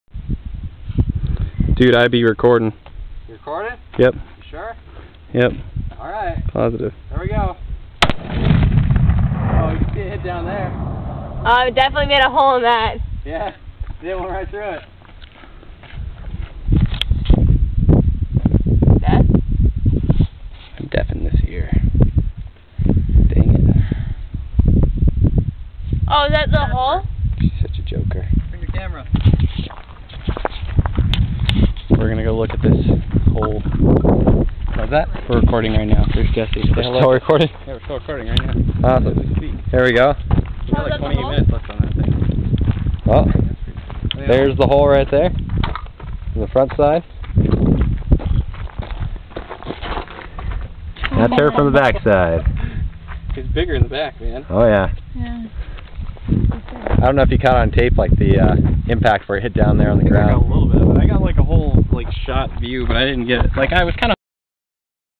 Black Powder Cannon 1.25" lead cannon balls
I machined this cannon and the cast for the cannonballs.  It packs a pretty good little punch.  The hardest part was the rifling in the bore.